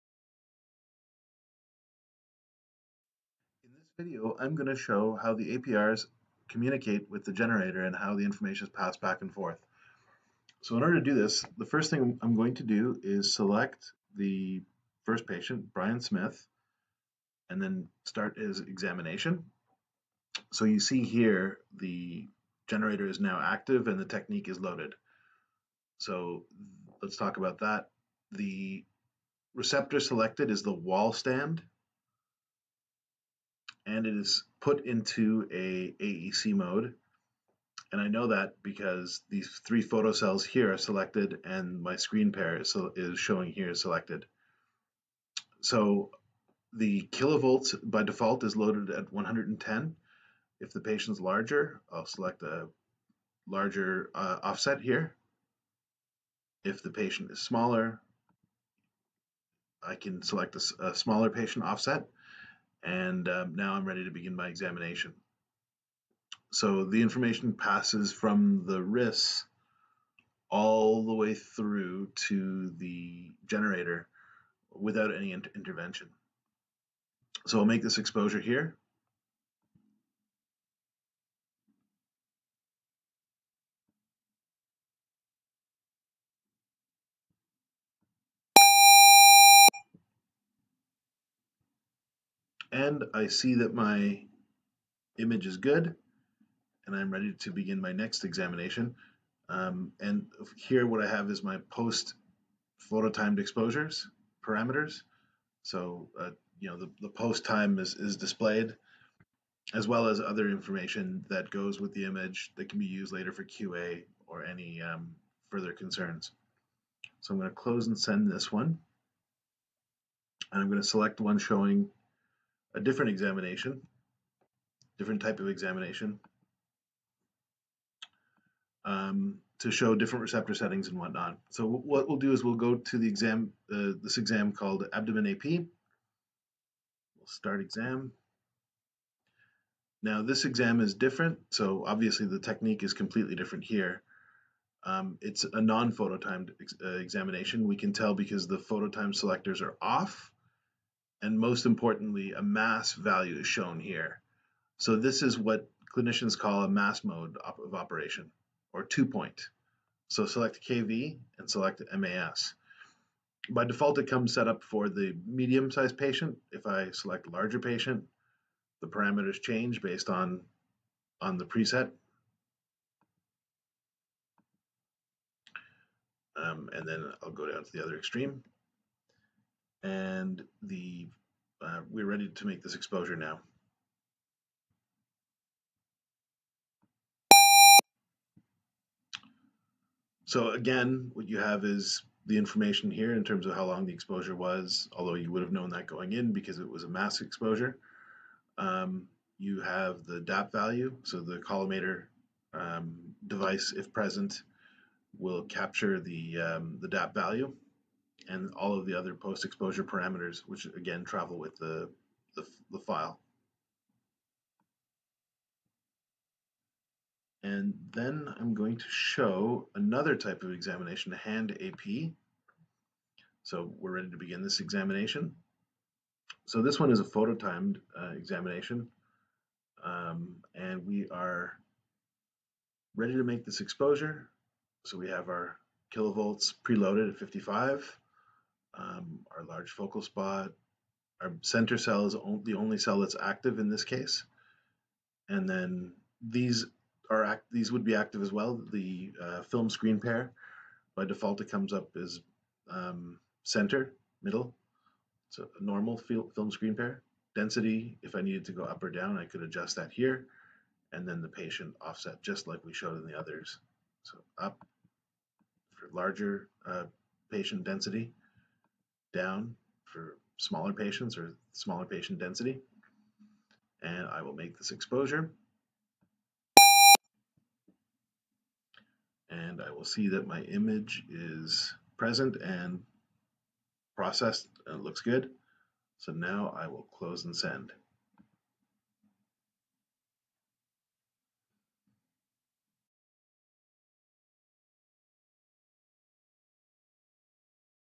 Spellman & AGFA Musica Acquisition Workstation Single Console Generator Integration
Demonstrating AGFA Musica APR Driven Generator Technique Settings